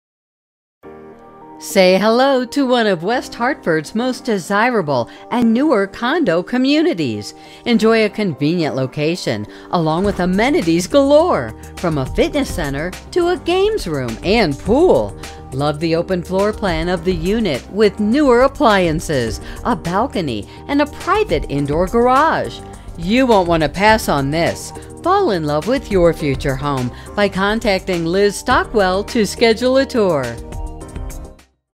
34 Schoolhouse Drive Unit 303 West Hartford, CT | ColdwellBankerHomes.com
34 Schoolhouse Drive Unit 303, West Hartford, CT

This location is one of West Hartfords most desirable and newer condo communities. This popular Elmwood neighborhood is just minutes away from West Hartford center with award winning restaurants and shopping. Commute to coffee shops and local restaurants.  Enjoy all Quaker Greens unparalleled first